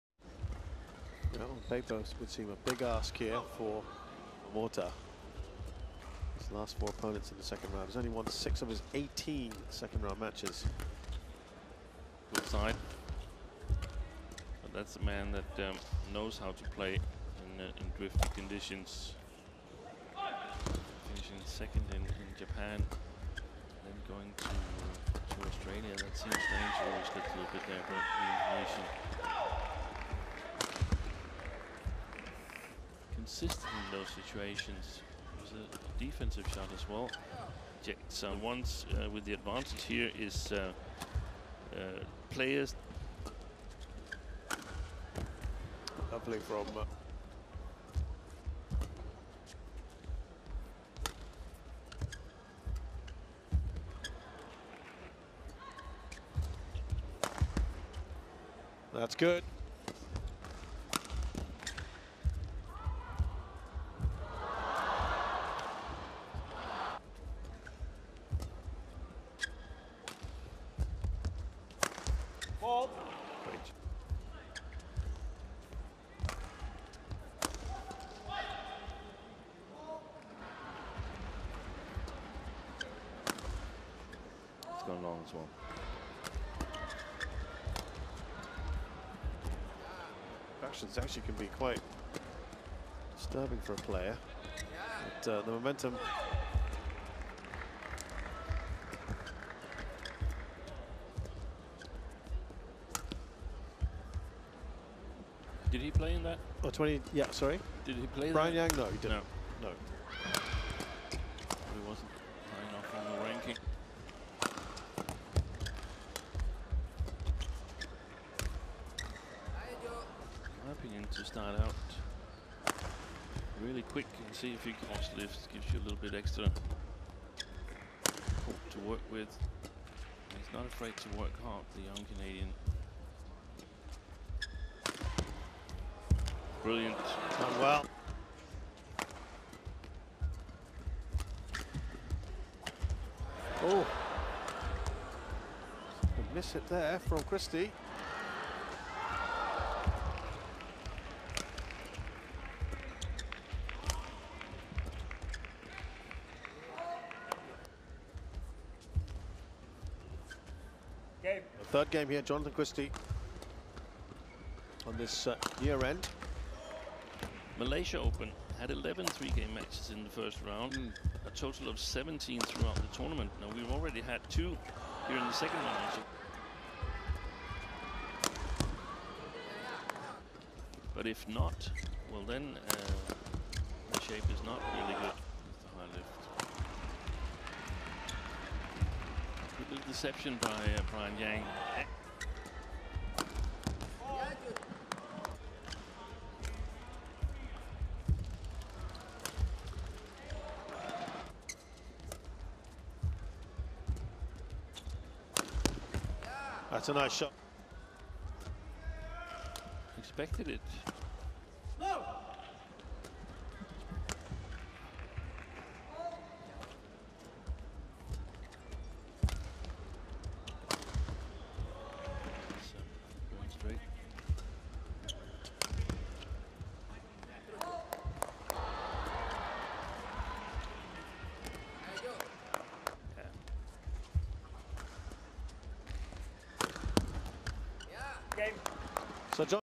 VICTOR China Open 2023 Round of 16 || Jonathan Christie (INA) vs Brian Yang (CND) || Come Back WIN
This channel is only for sharing information about Badminton, the latest updates on world badminton tournaments.